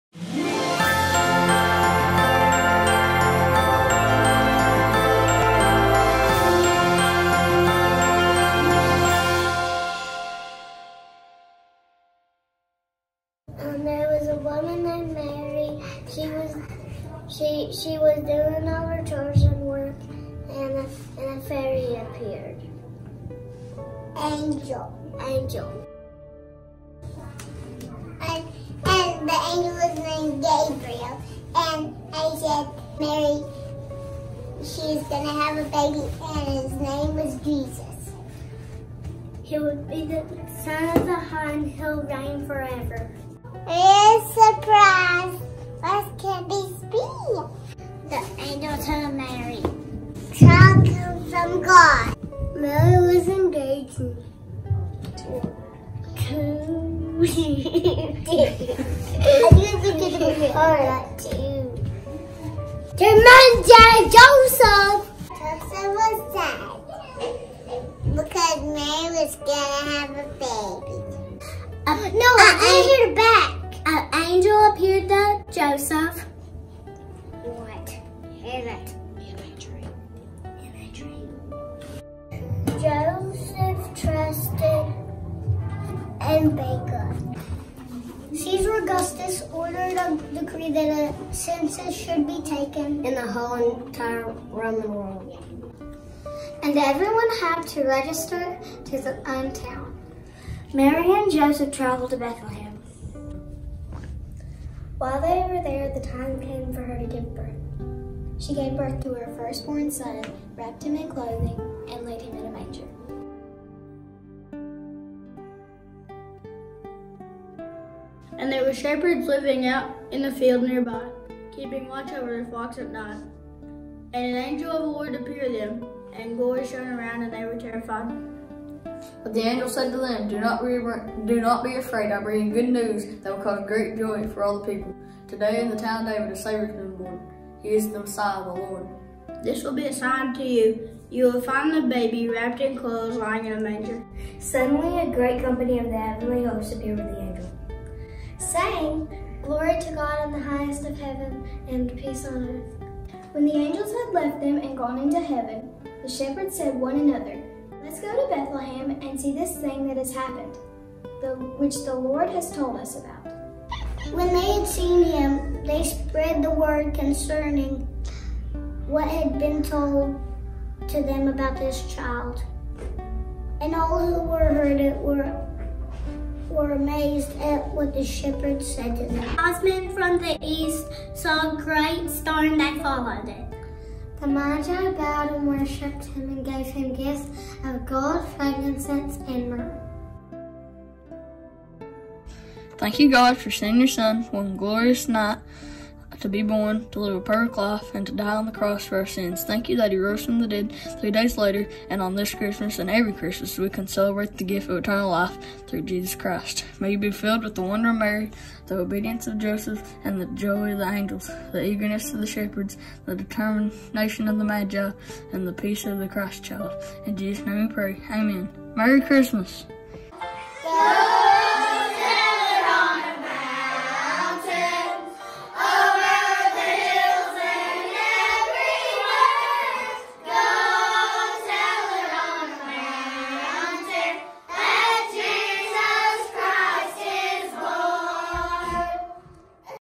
church christmas 1
Our children's church tell the bible story of the birth of jesus.